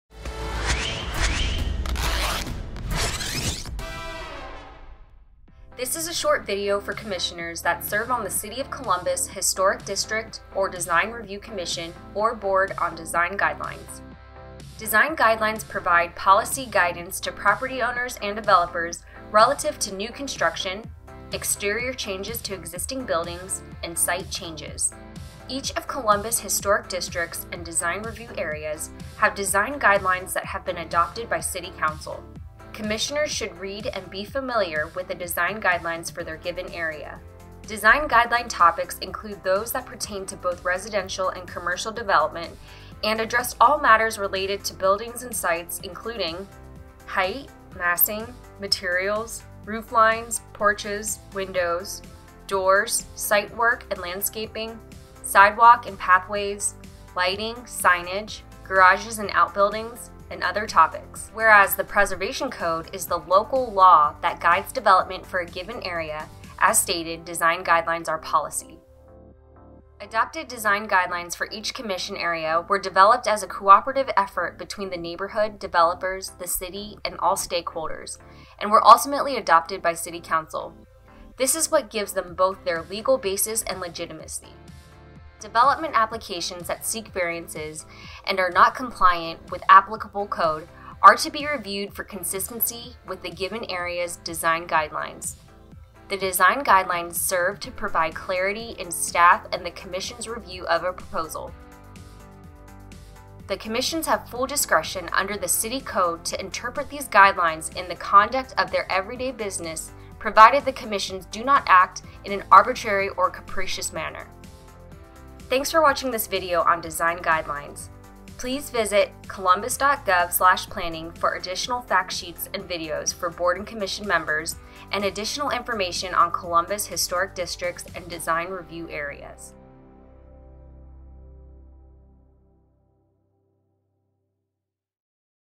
Design Guidelines
Design guidelines provide policy guidance to property owners and developers relative to new construction, exterior changes to existing buildings and site changes.  Each of Columbus historic districts and design review areas have design guidelines that have been adopted by City Council.  Commissioners should read and be familiar with the design guidelines for their given area.